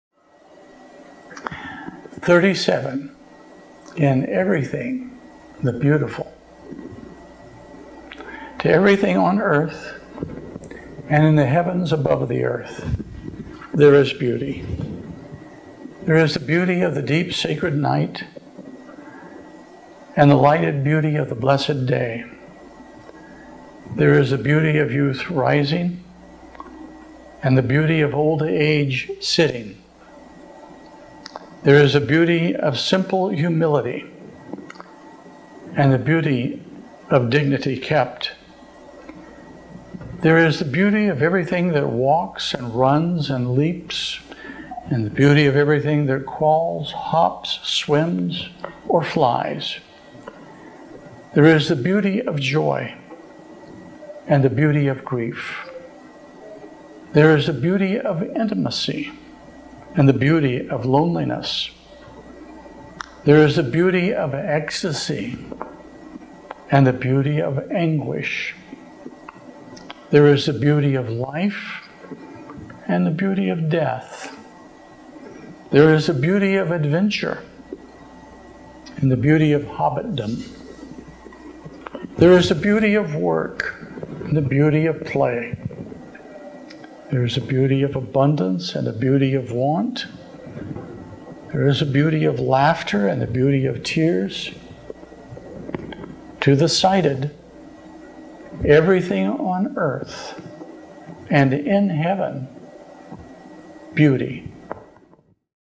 Father Larry Hart - Day 37 "In Everything - The Beautiful"
This is a special series of 47 daily Lenten Meditations.